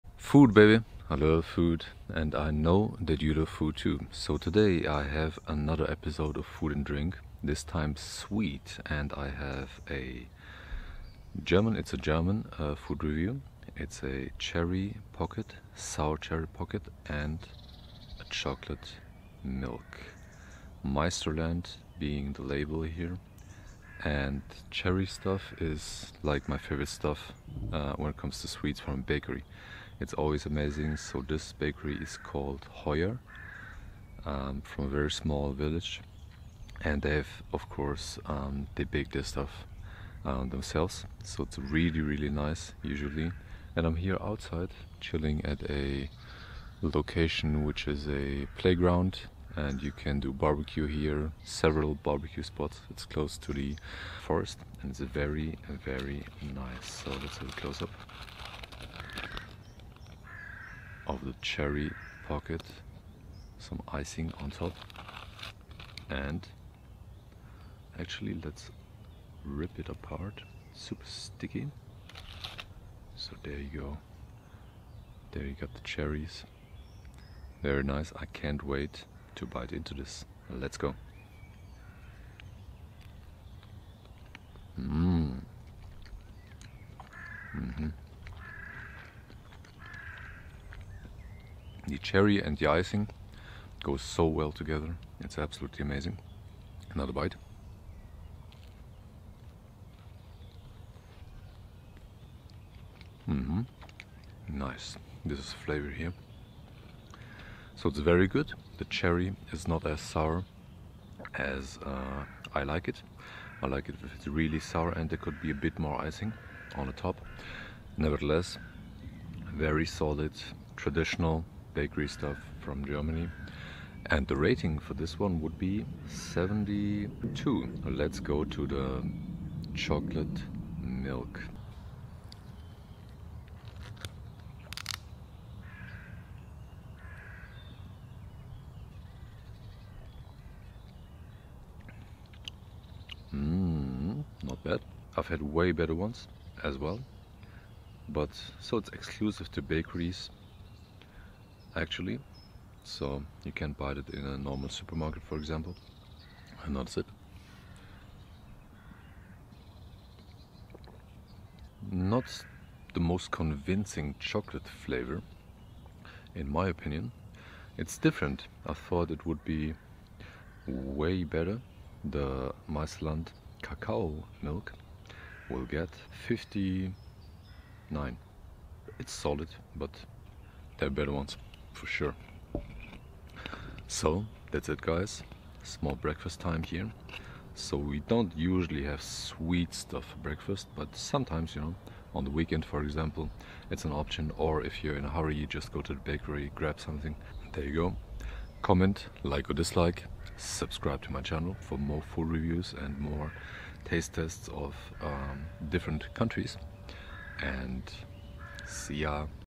German Breakfast Taste Test | Cherry Turnover & Chocolate Milk | Food & Drink
In this episode of Food & Drink you will see a sweet German breakfast: a yummy cherry turnover from a small bakery called Hoyler and a chocolate milk. Sweet pastries from German bakeries are called "sweet things" (Süße Stückchen). Love that name!

A nice and relaxed food review from the seventh of my twelve cheat days in 2017.